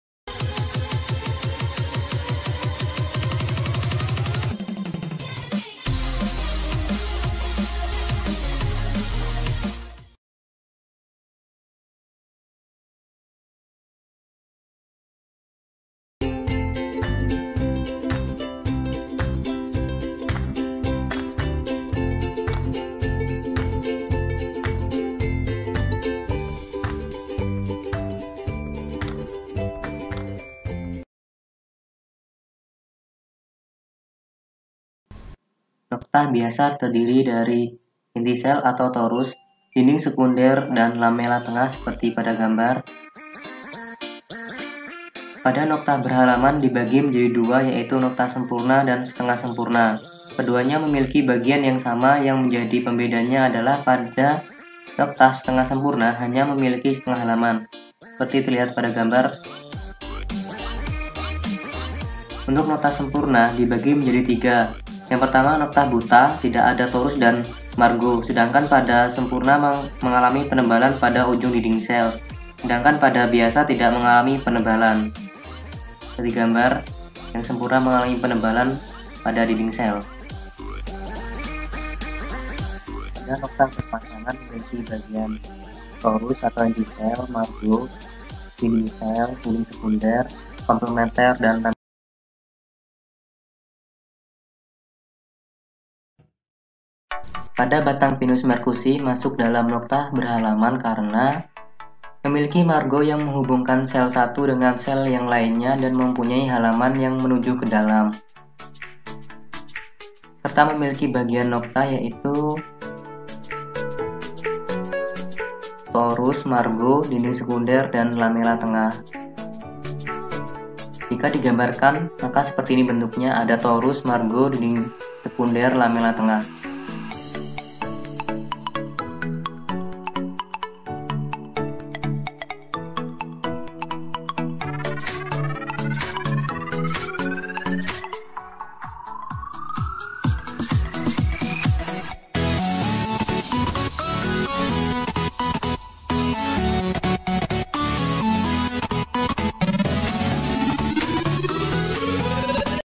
Noktah pada awetan batang pinus mercussi
Menjelaskan macam macam noktah pada tumbuhan contohnya pada awetan batang pohon pinus mercussi